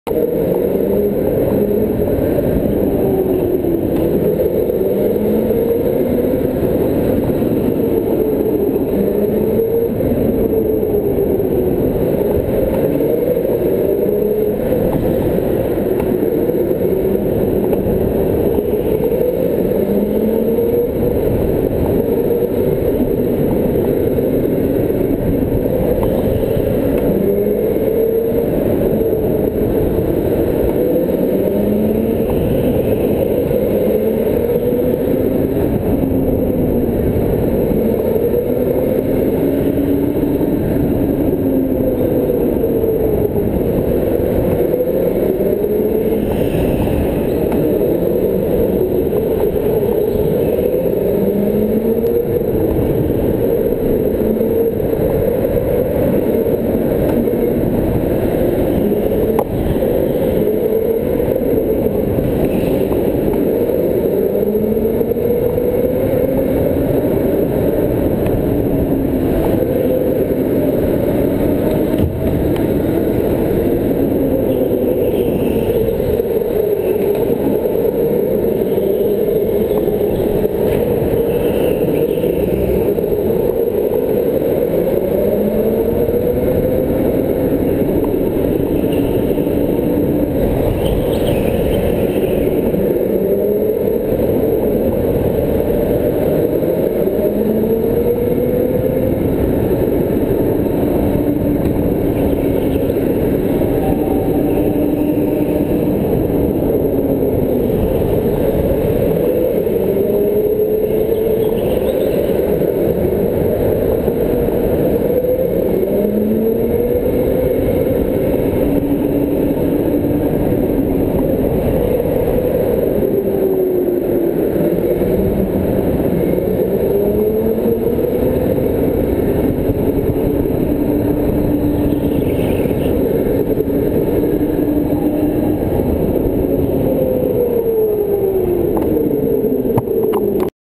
GO Kart Racing Go Pro HD Hero 720p
racing go karts at k1 racing. just testing how the gopro does with indoor lighting conditions